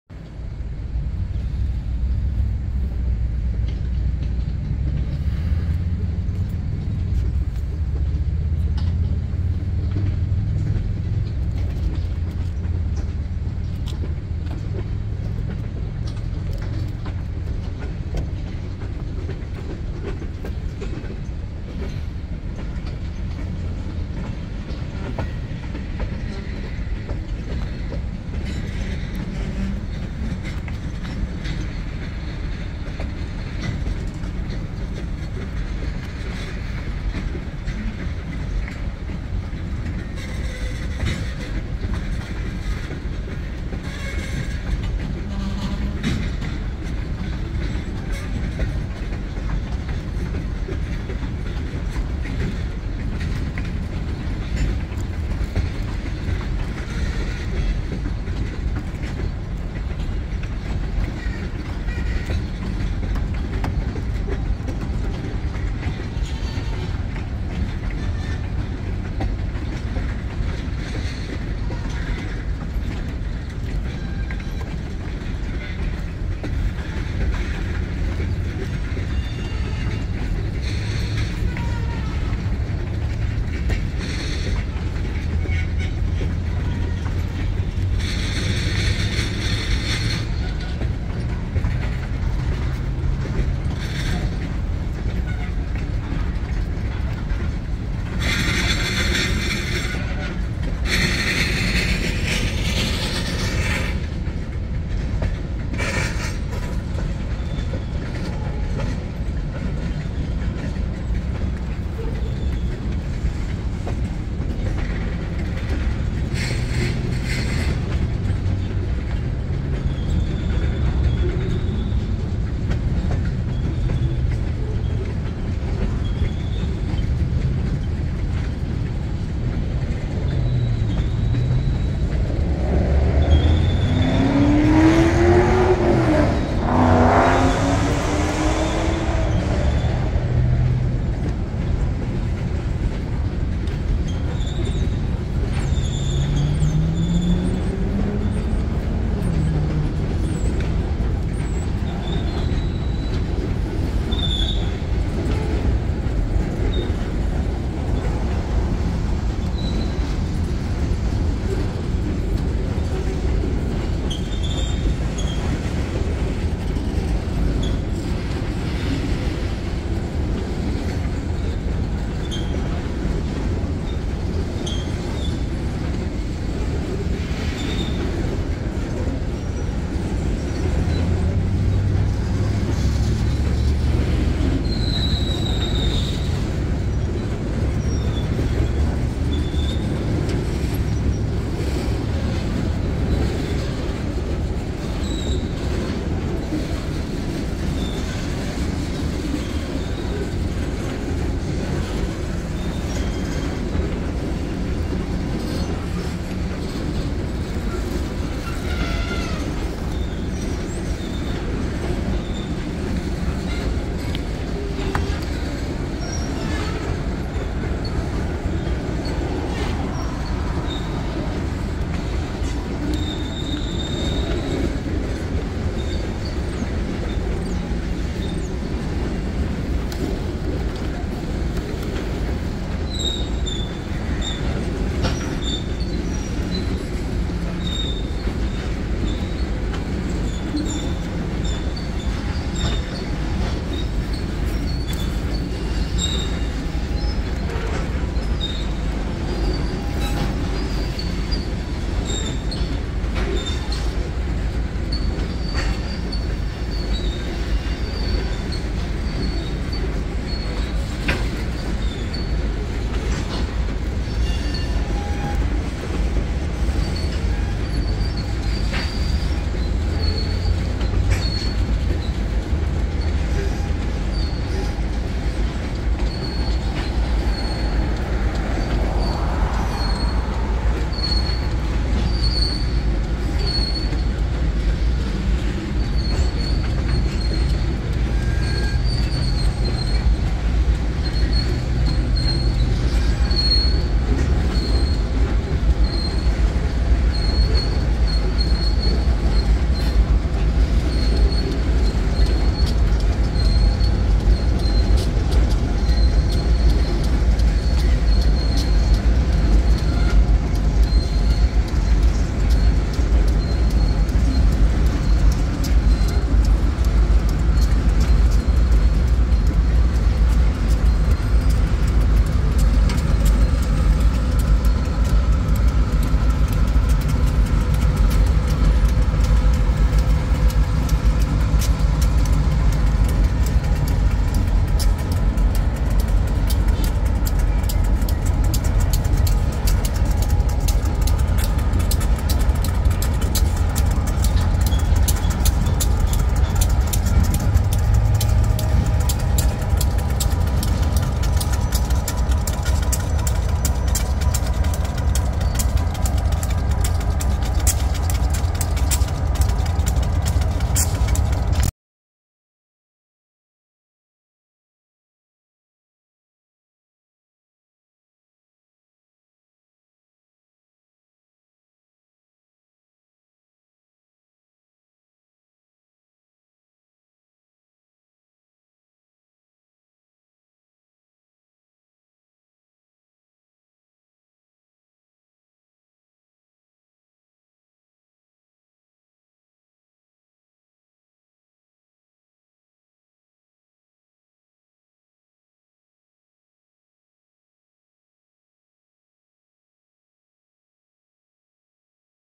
Lima, OH: Part 2 CFW&E returns. Stay until end to see a funny story of this video.
This is part 2 of this train where it returns to the yard to pick up more rail cars.